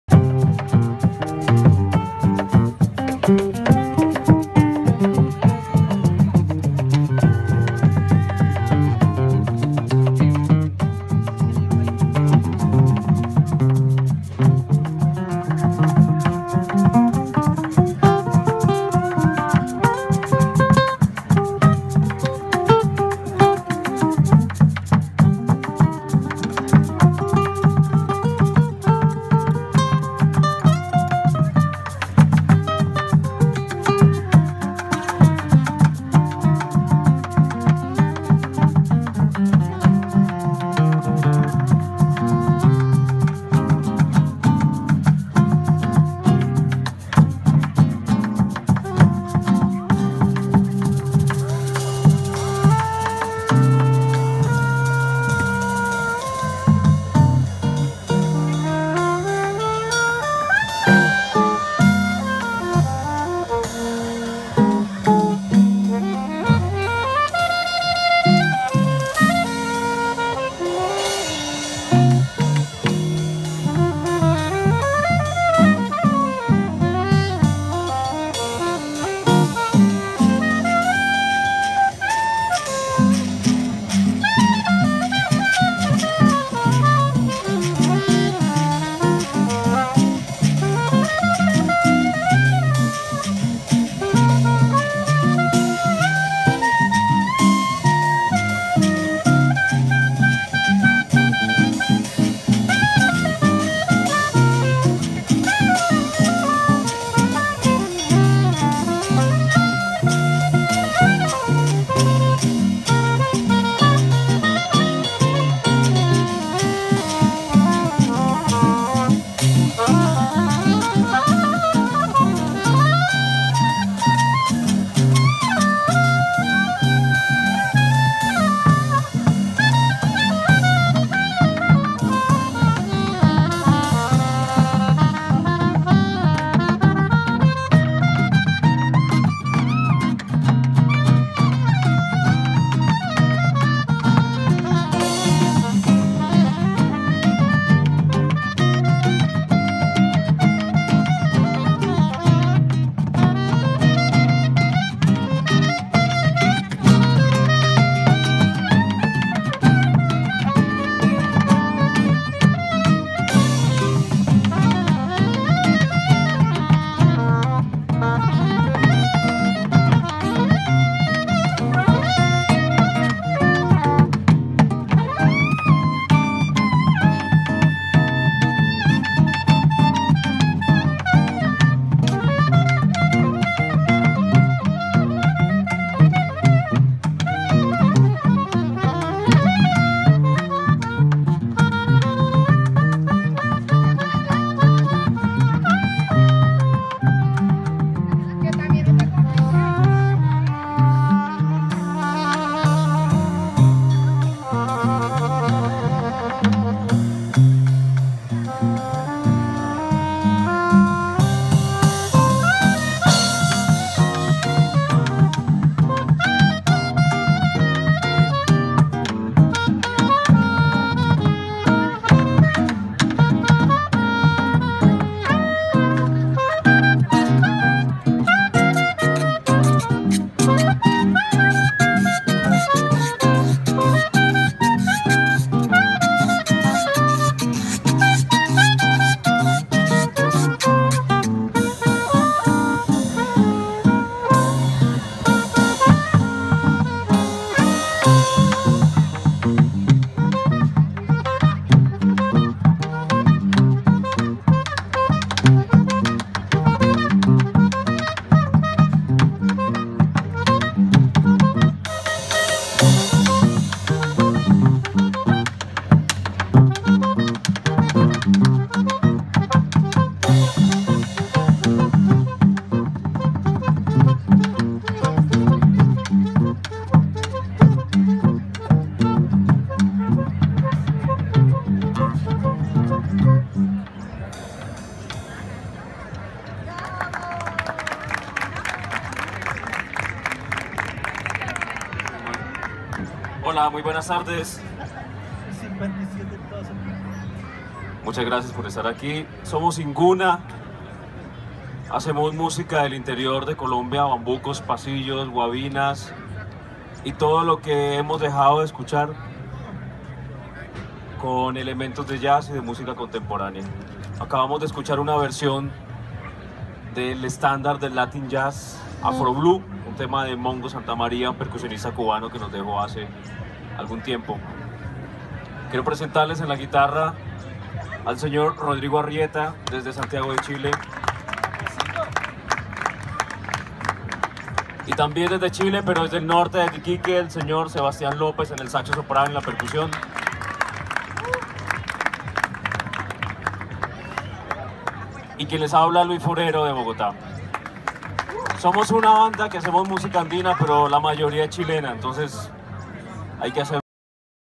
Sendero Artístico y cultural Alimentarte 2017 programa T de Teatro
Los artistas sendero artístico y cultural de Alimentarte 2017 son programados por T de Teatro.
En el video, Inguna trío y su música colombiana jazz. Algo va bien con las artes escénicas en Bogotá.